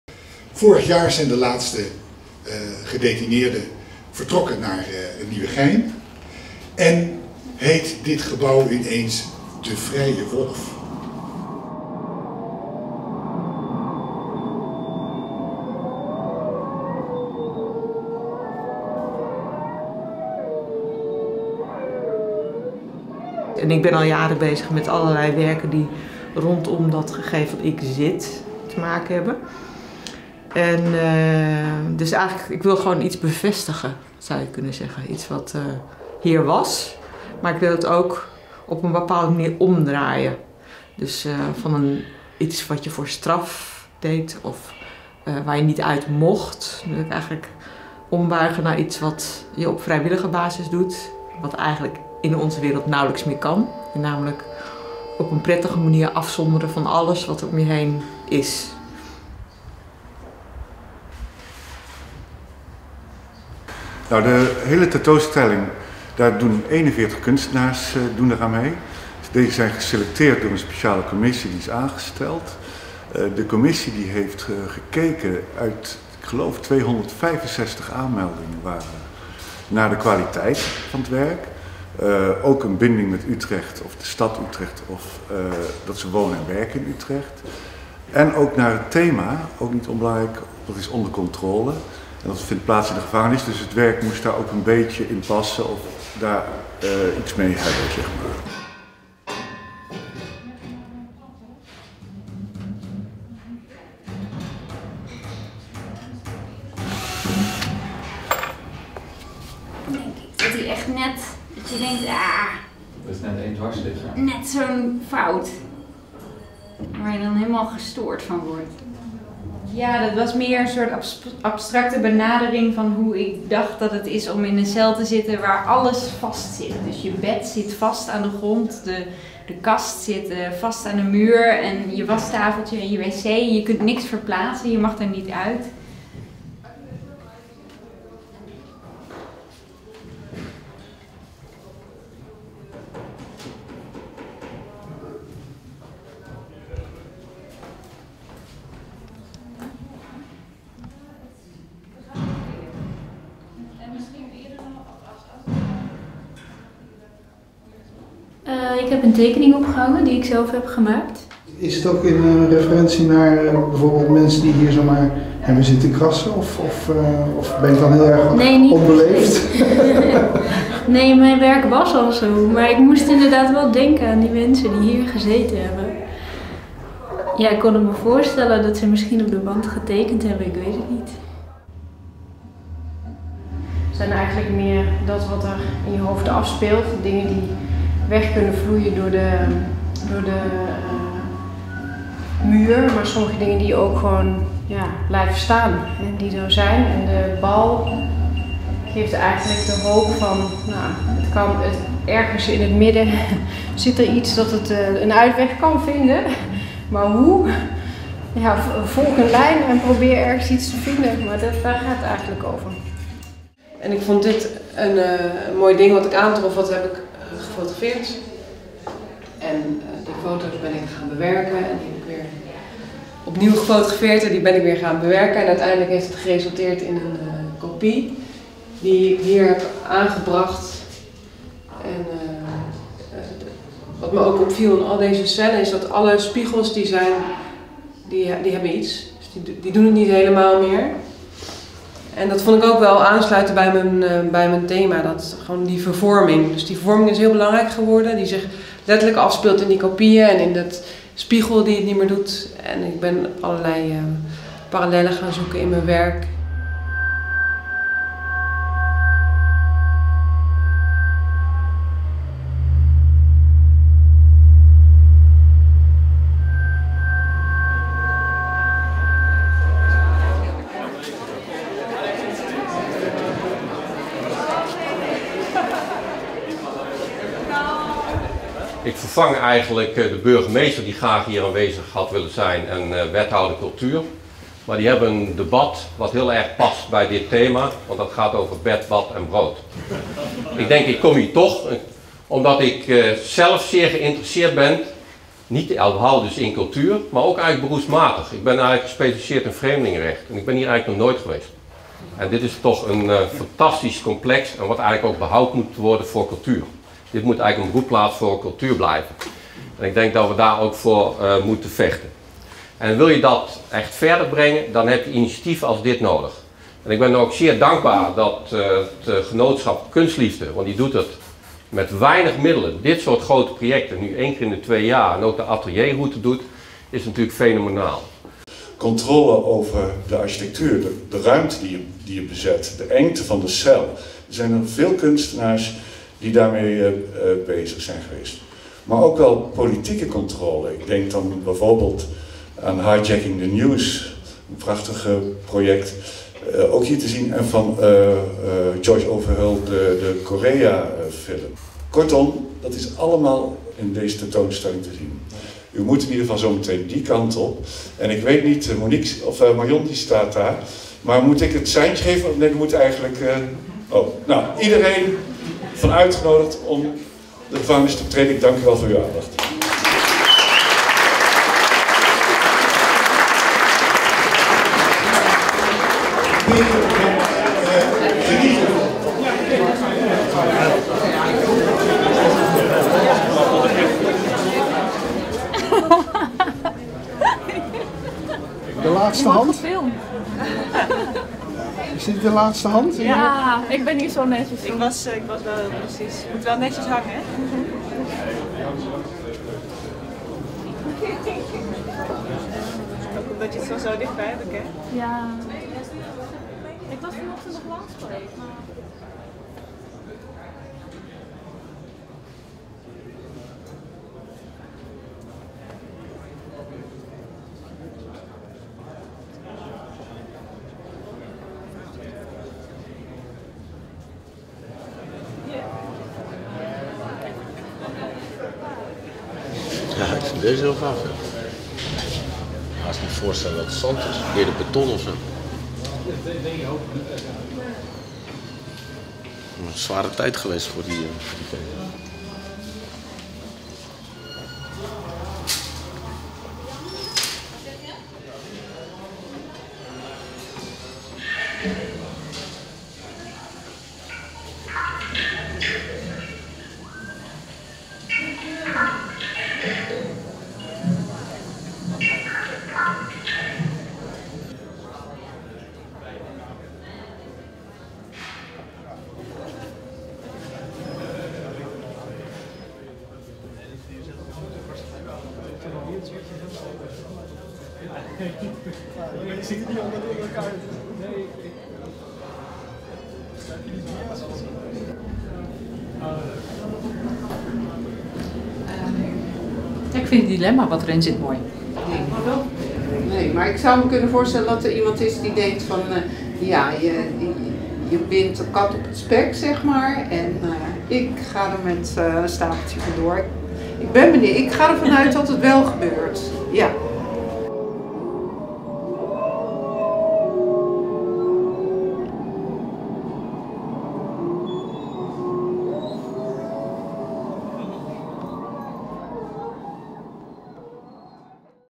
UtrechtDownUnder Onder Controle
Op 1 mei is de tentoonstelling UtrechtDownUnder ONDER CONTROLE geopend; actuele beeldende kunst van 41 geselecteerde Utrechtse kunstenaars. De hele maand mei wacht bezoekers een avontuurlijke kunstroute in cellen, aangrenzende werkruimten, de kerk en luchtplaatsen in de voormalige gevangenis aan het Wolvenplein te Utrecht.
De tentoonstelling is te bezichtigen van 1 t/m 31 mei 2015 van woensdag t/m zondag van 13.00 uur - 18.00 uur